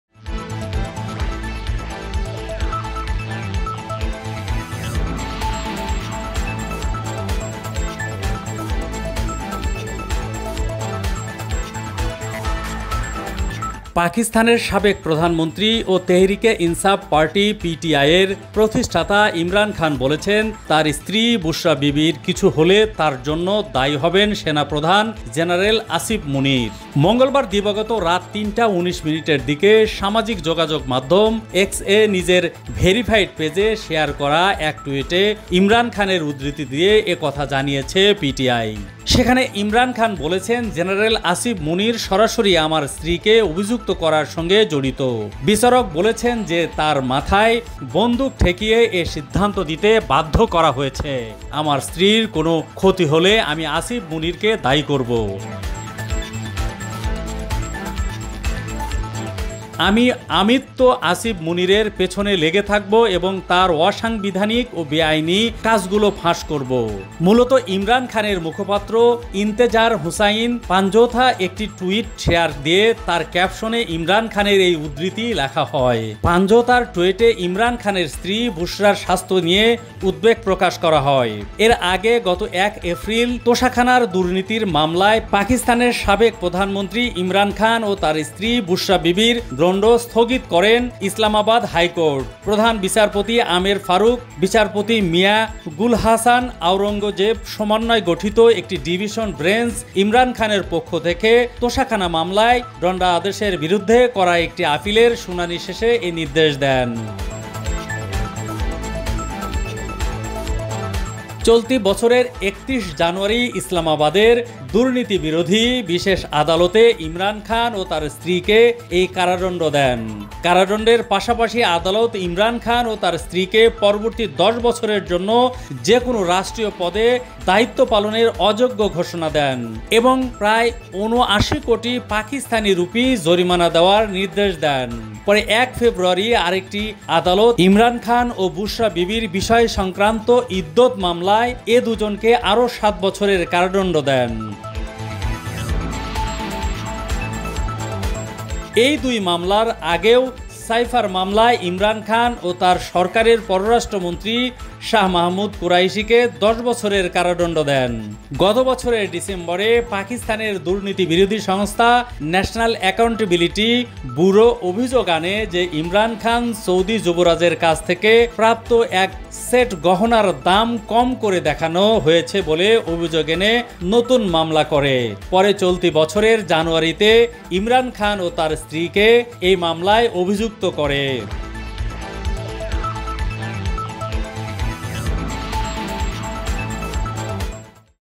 স্ত্রী বুশরাকে নিয়ে সেনাপ্রধানকে যে হু*ম'কি দিলেন ইমরান খান | Pundro Tv News
Report Name: স্ত্রী বুশরাকে নিয়ে সেনাপ্রধানকে যে হু*ম'কি দিলেন ইমরান খান | Pundro Tv News
Reporter: